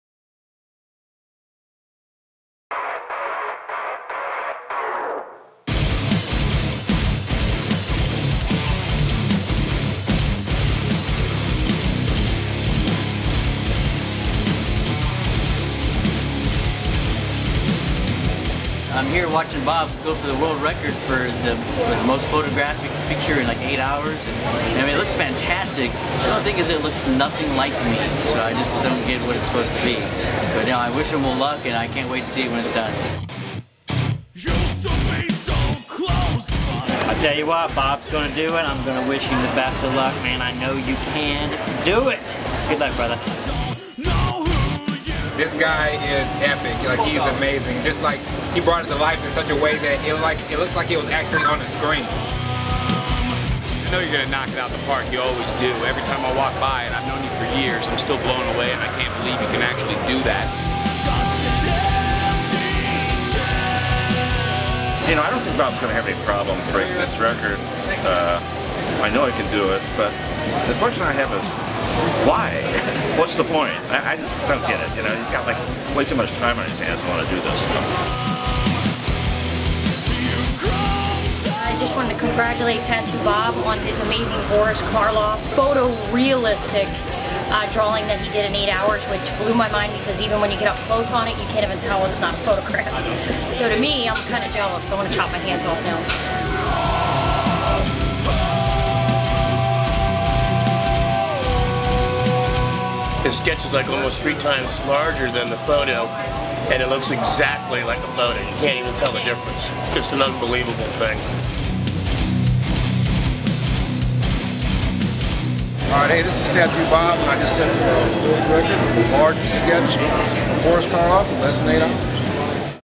Tattoo Bob's Guiness World Record At the Full Moon Tattoo & Horror Fest
Tattoo artist Tattoo Bob goes for a world record by sketching the largest photorealistic portrait of Boris Karloff as the Frankenstein monster.